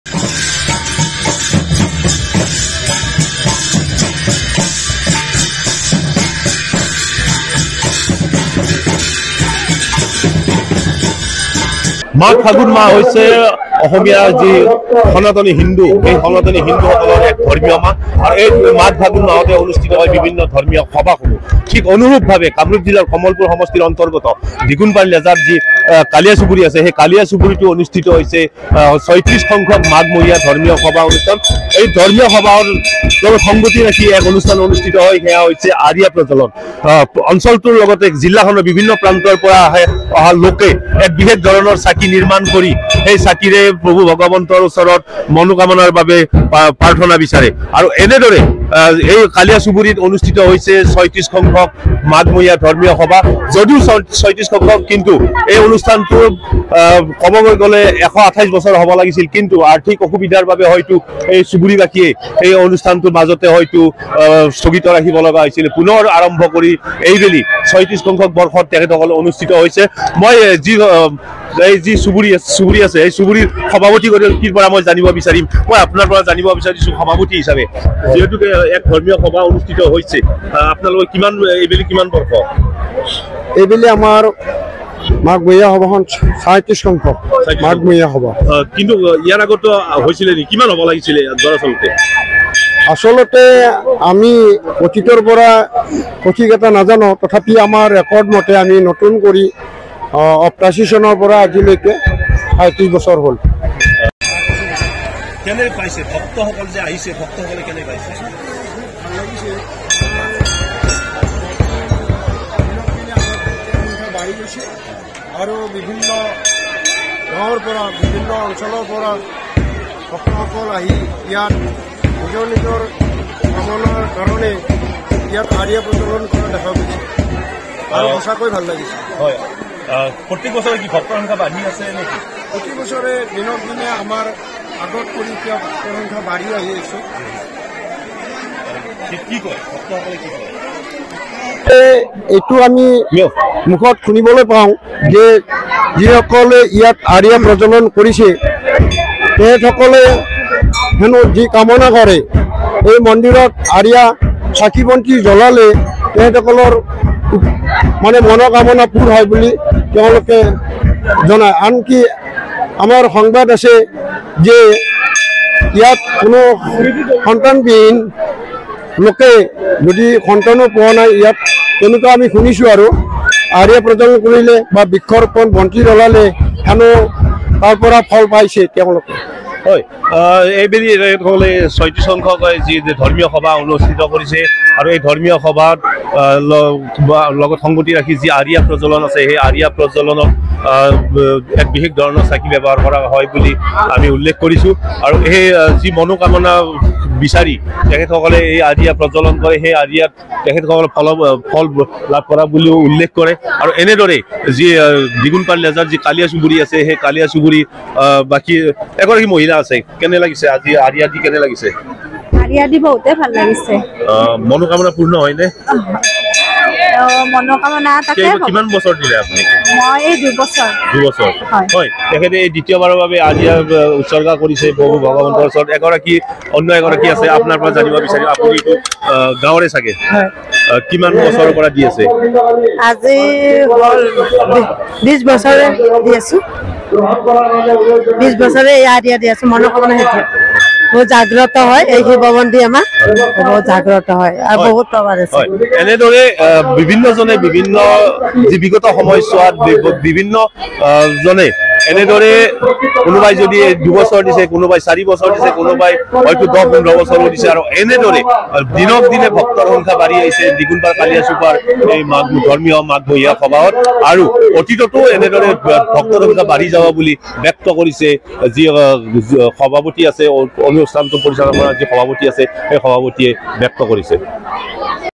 Baihata News : কালিয়া চুবুৰিটো দেখা গ'ল ৩৬ সংখ্যক মাঘ মহীযা সবাহৰ উখল মাখল পৰিবেশ | N18V
Baihata News | মাঘ বা ফাগুণ  মাহ অসমীয়া হিন্দু সনাতনি ধৰ্মীয়  সকলৰ ধৰ্মীয় মাহ । মাঘ বা ফাগুণ মাহতে অনুষ্ঠিত হয় বিভিন্ন ধৰ্মীয় অনুষ্ঠান । যাৰ বাবে মাঘ বা ফাগুণ মাহক মাধৱৰ মাহ বুলিও উল্লেখ কৰে । মাঘ বা ফাগুণ মাহত  নামনি অসমত অনুষ্ঠিত হয় বিভিন্ন সবাহ মহোৎসৱ । অনুৰূপ ভাবে অবিভক্ত কামৰূপ জিলাৰ বিভিন্ন মন্দিৰ  , সত্ৰ আদিতো অনুষ্ঠিত হয় সবাহ মহোৎসৱ।  কামৰূপ জিলাৰ কমলপুৰ সমষ্টিৰ অন্তৰ্গত দিগুণপাৰ লেজা গাঁৱৰ কালিয়া চুবুৰিটো দেখা গ'ল ৩৬ সংখ্যক মাঘ মহীযা সবাহৰ উখল মাখল পৰিবেশ । কলগছৰ গুৰিৰে নিৰ্মিত বিশেষ ধণৰৰ চাকি নিৰ্মাণ কৰি পূজা আগবঢ়ালে  দেৱাদিদেৱ মহাদেৱক । এইদৰে পূজা অৰ্চনা কৰিলে মনোবাঞ্চা পূৰ্ণ হয় বুলি দৃঢ় বিশ্বাস মানস সমাজত ।  এই বিষয়ত আমাৰ এক বিশেষ প্ৰতিবেদন